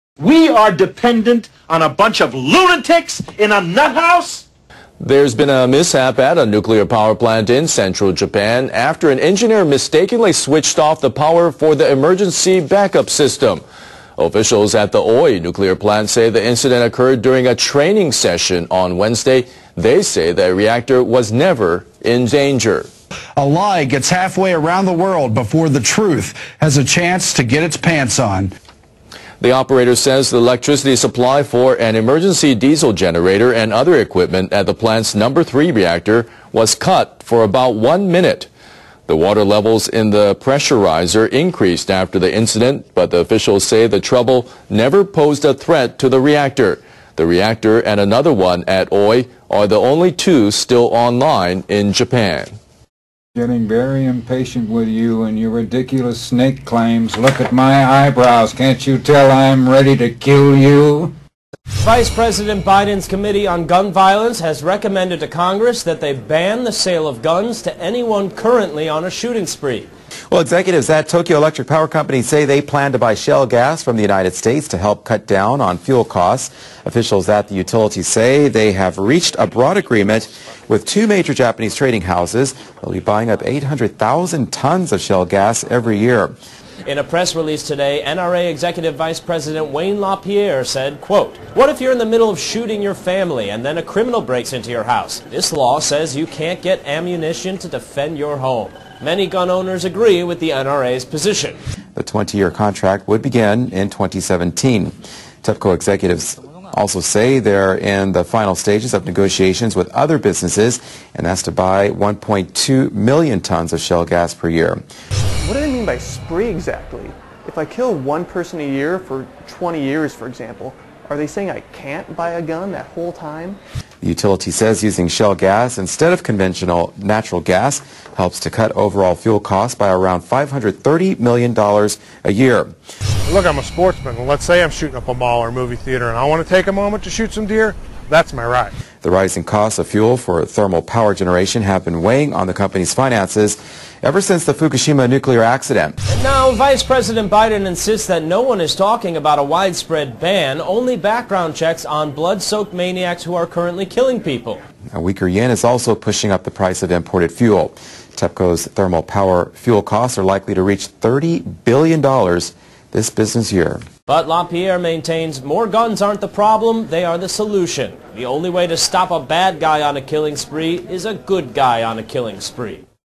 Japan's Epidemic Nuclear FAIL: Fukushima update 2/6/13
Human error briefly stops Ohi plant power supply

YouTube Just sent me this message: Your settings could not be saved: The description is too long or contains illegal characters.  LOL doesn't work for them?

Japan's Nuclear Regulation Authority says human error briefly stopped the emergency power supply at the Ohi power plant in Fukui Prefecture on Wednesday. The incident caused no problems for the operation of the plant.

Officials from the authority say the electrical supply stopped for about a minute from a battery necessary to control the emergency diesel generator and the motor valve at the plant's Number 3 reactor. The reactor is one of two at the Ohi plant that are online in Japan. All other reactors in the country are offline.

The officials said a reactor operator mistakenly pressed a switch that shuts off power during a training event by Kansai Electric Power Company, the plant's operator.

Power was restored immediately after the switch was turned on.

The incident caused a brief increase in water levels in the pressurizer for the cooling water. The officials said the trouble did not affect the safety of the reactor.

The officials are looking closely into the details of the incident.

Kansai Electric reported the incident to the central and local governments. It was the first breach of nuclear safety regulations since the utility resumed operation last July after the Fukushima accident in 2011.

TEPCO to buy US shale gas

Tokyo Electric Power Company says it plans to buy shale gas from the United States to help cut down on fuel costs.

The utility says it has reached a broad agreement with 2 major Japanese trading houses to purchase 800-thousand tons of shale gas every year. The trading houses will buy from the US.

The 20-year-contract would begin in 2017.

TEPCO also says it's in the final stages of negotiations with other businesses to purchase 1-point-2-million tons of shale gas per year.

The utility says using shale gas, which is somewhat cheaper than conventional natural gas, would help cut overall fuel costs by around 530-million dollars a year.

Since the nuclear accident at the utility's Fukushima Daiichi power plant, rising costs of fuel for thermal power generation have been weighing on the company's finances. Thermal power fuel costs are likely to reach around 30-billion dollars this business year.

NRA to set toughest nuclear plant safety rules

The chairman of Japan's Nuclear Regulation Authority says it will set the world's toughest safety standards for nuclear power plants.

Shun-ichi Tanaka made the remark at a committee hearing of the Upper House of the Diet on Wednesday.

Tanaka said he's been reflecting on the nuclear disaster at the Fukushima Daiichi power plant and working to protect people and the environment as his mission. He added that he's been making decisions from scientific and technological viewpoints by ensuring transparency.

Tanaka also referred to new nuclear plant safety standards that his agency is to set by July. He said he plans to dispatch agency officials to international bodies this month to hear their opinions.

He said Japan's rules are not as strict as global ones and that he aims to set the world's highest standards based on lessons learned from the Fukushima accident.

Tanaka also commented on differences in assessments of the NRA's geological survey results at nuclear plants by the agency and plant operators.

He said if operators are not convinced by the agency's judgments, they should gather data themselves to scientifically argue their assessments at meetings that the NRA will set up.

Tanaka was appointed by former Prime Minister Yoshihiko Noda in September but must be approved by both Diet chambers.

S.Korea: North may be readying hydrogen bomb

South Korea's top military commander says North Korea may test a massive weapon as a stage in completing a hydrogen bomb.

The chairman of the Joint Chiefs of Staff, General Jung Seung-jo, was speaking to the parliament's defense committee on Wednesday.

Jung said the South Korean military would not exclude the possibility that the North would detonate a boosted fission weapon, a smaller and more sophisticated nuclear bomb.

A boosted fission bomb has greater explosive power than a conventional atomic bomb, with increased frequency of nuclear fission.

Jung said North Korea may conduct a bomb test using high-enriched uranium, or it could conduct two nuclear tests simultaneously.

The general said that North Korea's missile technology must have reached a significant level after the past two nuclear tests.

He said the North must be able to make a nuclear warhead small enough to be mounted on a missile.

Just FYI, the NRC in Japan has been changed to the NRA. LOL...
I cut short the lengthy update to bring you only the highlights.